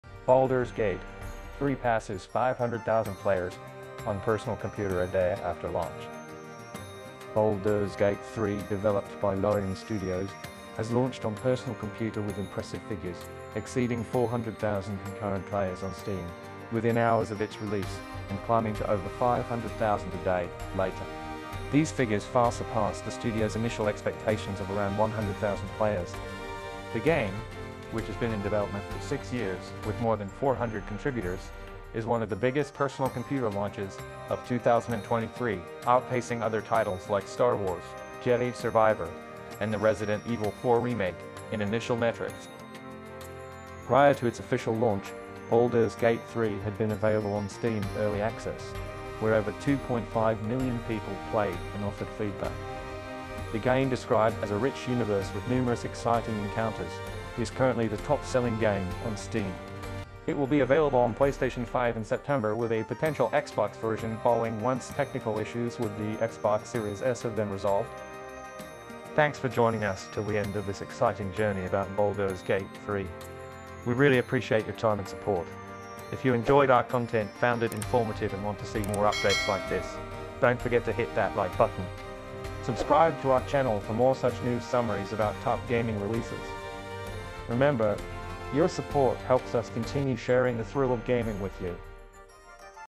Unprecedented Launch: Baldur's Gate Three Surpasses 500K Players on PC in 24 Hours!
Welcome to our latest video where we explore the incredible success of Baldur's Gate Three, brought to us by the renowned Larian Studios. This highly anticipated game has taken the Personal Computer gaming world by storm, surpassing expectations with its unprecedented player count.

Within hours of launching, the game soared to four hundred thousand concurrent players on Steam, a figure which escalated to over five hundred thousand just a day later. This incredible response far exceeded the developers' original expectations, who had estimated around one hundred thousand players at launch.

This video delves into the six-year long journey of the game's development, a process that involved more than four hundred dedicated contributors. Their hard work and effort have culminated in one of the biggest Personal Computer launches of 2023. The game's initial success outstrips other popular titles like Star Wars Jedi: Survivor and the Resident Evil Four remake, a testament to its captivating gameplay and immersive universe.

An important milestone in the game's journey has been its availability on Steam Early Access. Here, over two point five million people were given the opportunity to experience the game firsthand and offer invaluable feedback. This interactive process has been instrumental in shaping the game into its current form, a rich universe teeming with exciting encounters.

Currently, Baldur's Gate Three is the top-selling game on Steam, an achievement that speaks volumes about its popularity and mass appeal. And there's more to come. PlayStation Five users can look forward to experiencing this thrilling universe in September, with a potential Xbox version on the horizon once certain technical issues with the Xbox Series S have been resolved.

Join us in this video as we delve into the fascinating world of Baldur's Gate Three, exploring the intricacies of its gameplay, the challenges and successes of its development, and the overwhelming response it has received from gamers around the globe. Whether you're a seasoned gamer or new to the world of Personal Computer gaming, this video offers insights and perspectives that will enhance your gaming experience.

So, gear up for a thrilling journey into the world of Baldur's Gate Three, a game that has truly redefined the landscape of Personal Computer gaming.